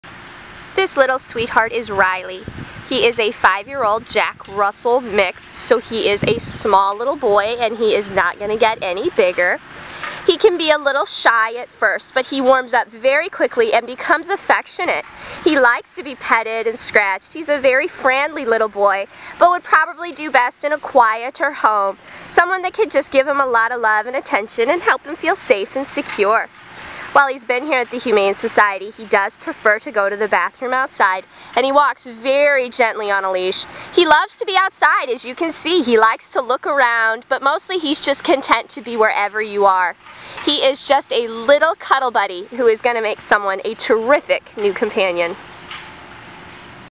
Riley-Available at the Humane Society of Missouri
RILEY - ID#A504838

I am a neutered male, white Parson (Jack) Russell Terrier mix.
I have been at the shelter since Apr 04, 2012.